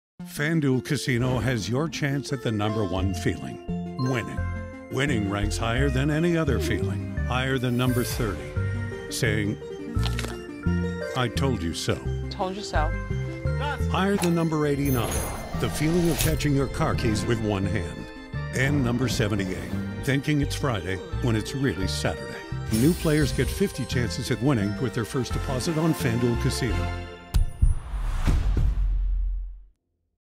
FanDuel Casino - Told You So
Agency  : Unknown
Campaign : “Told You So” 50 Bonus Spins
Advertiser : FanDuel Casino
Brand  : FanDuel
Length  : 30 Seconds
Posted   : Sept 26, 2023

In this Movie Ad we’ll walk you through:
Highlights the feeling of winning as the best feeling in the world. It compares winning to other great feelings, such as catching your car keys with one hand and thinking it's Friday when it's really Saturday.

TIMESTAMPS :
00:00 FanDuel Casino - Told You So
Plot :
The ad then encourages viewers to try FanDuel Casino, where new players can get 50 chances of winning with their first deposit.